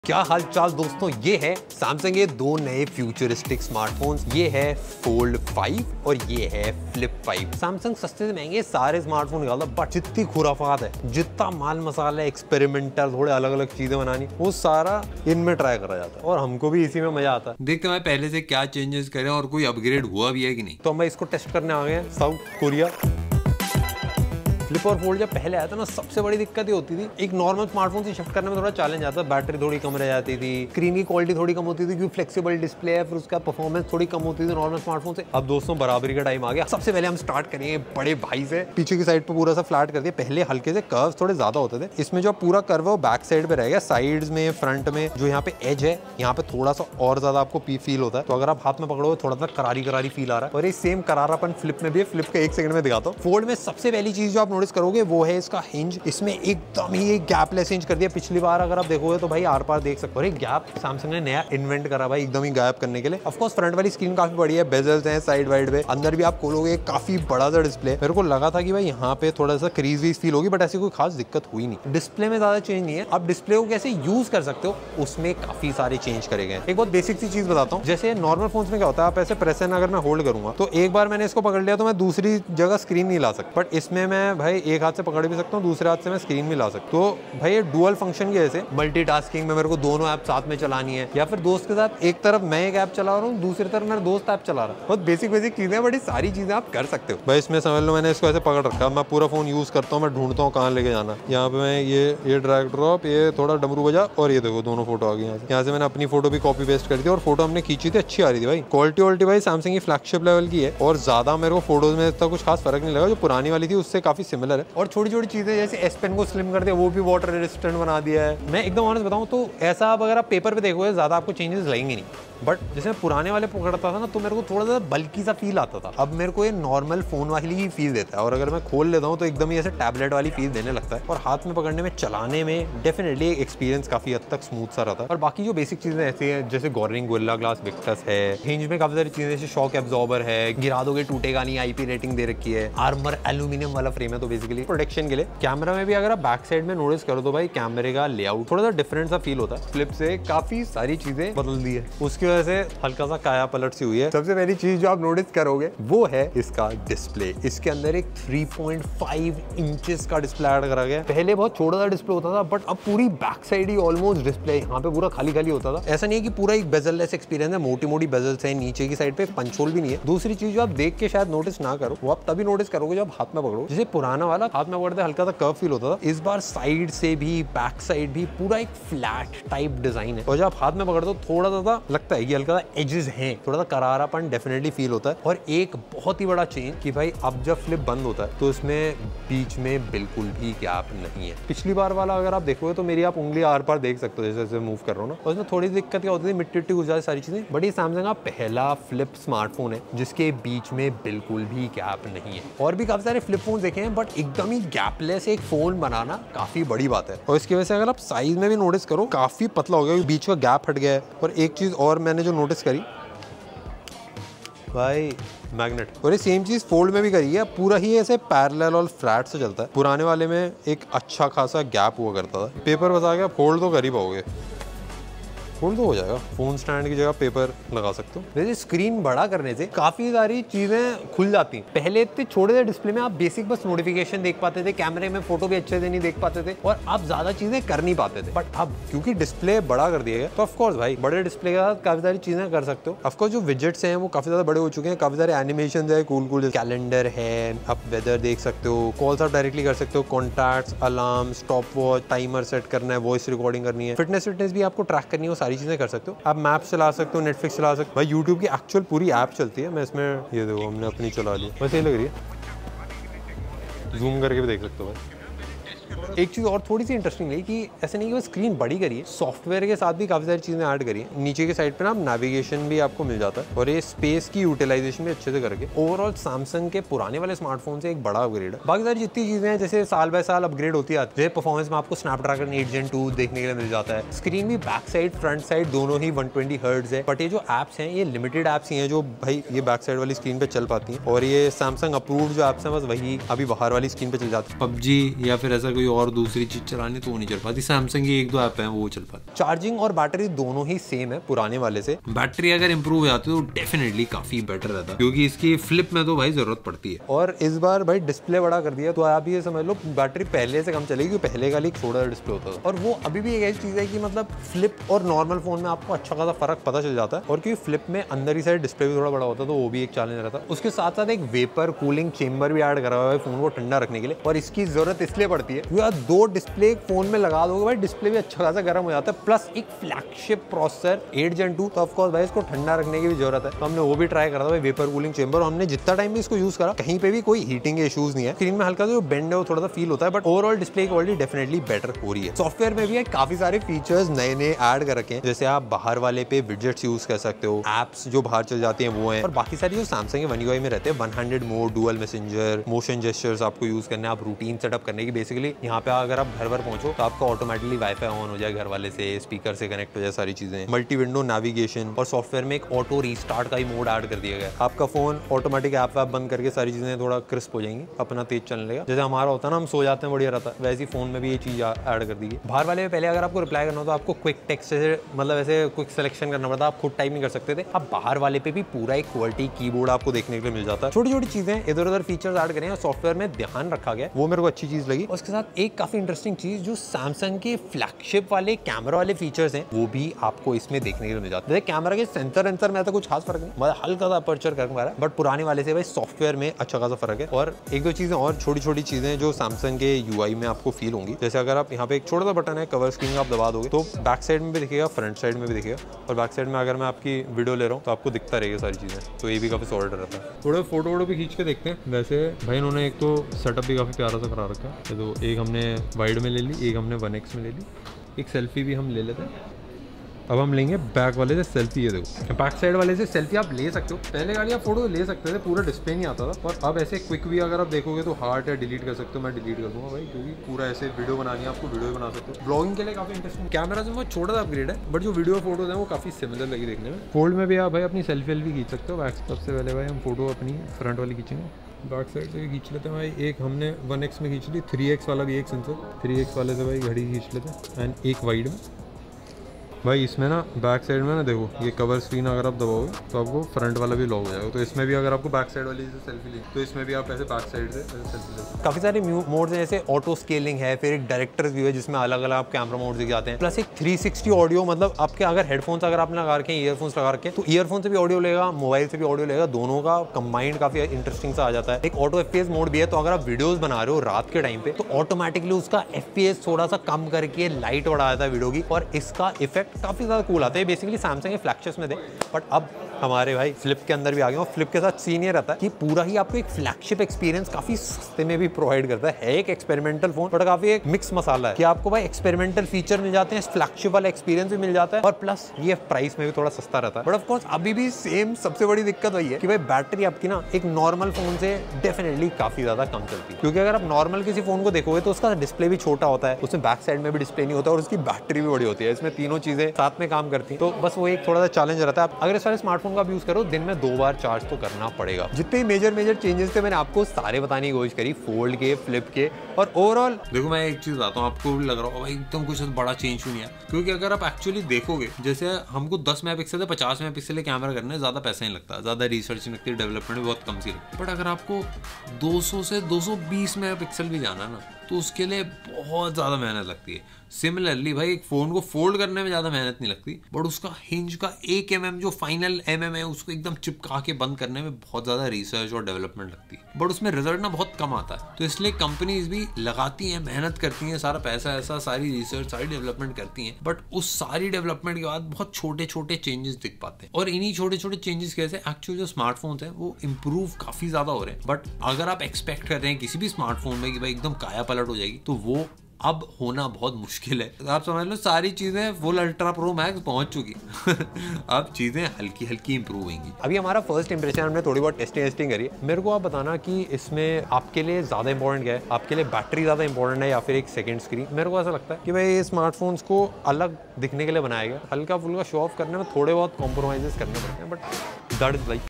Samsung Z Fold 5 & Z Flip 5 First Impressions - What's New ?
Samsung ke naye foldables aa gaye hai doston !